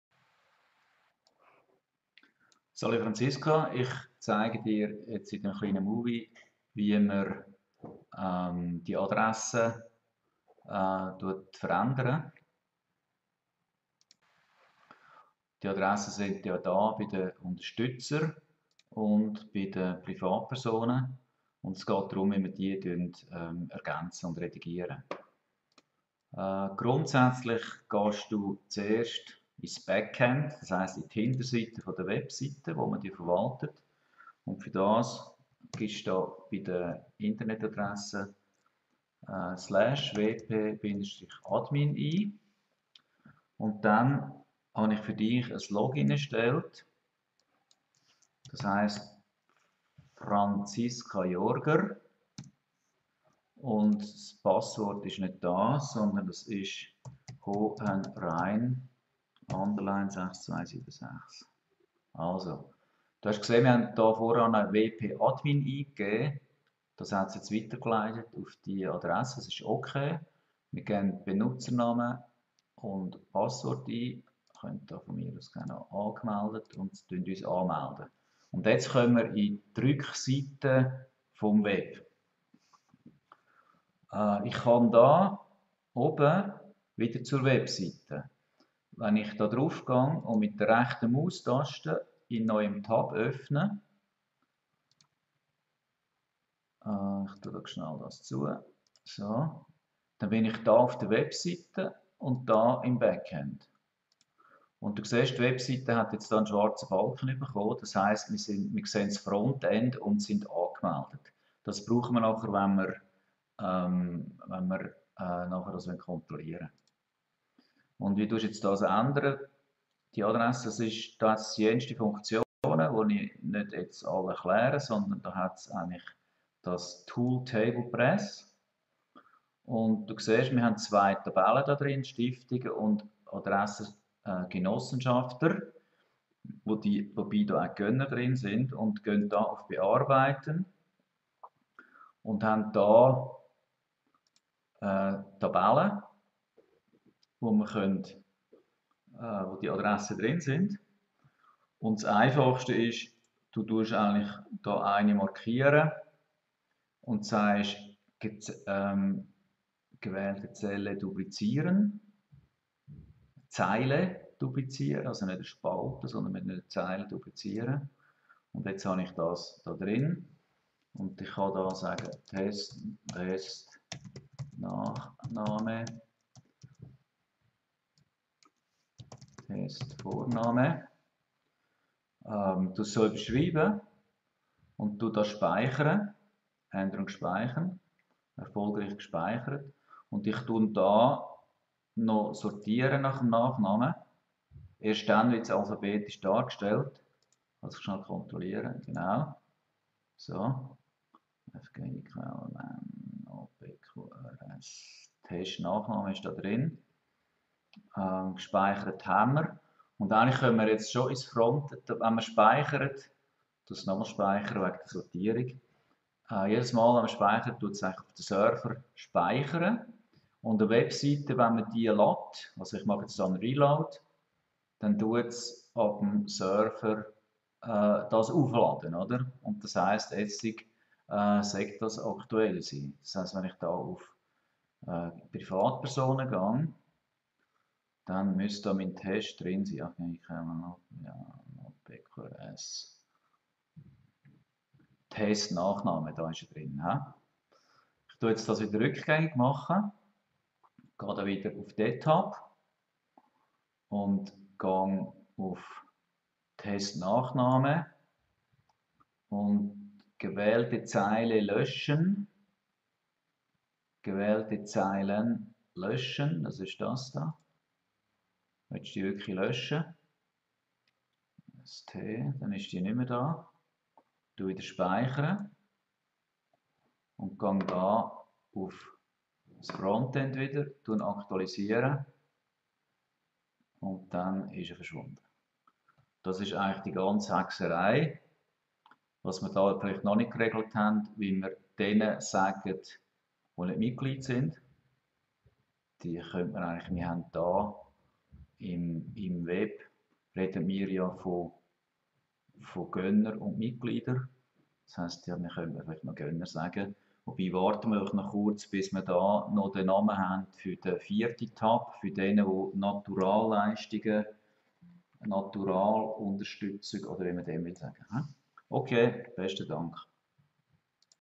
im Chrüz: Schulungsvideo Genossenschafts- und Gönnerliste redigieren
Zeigt wie neue Mitglieder und Gönner erfasst werden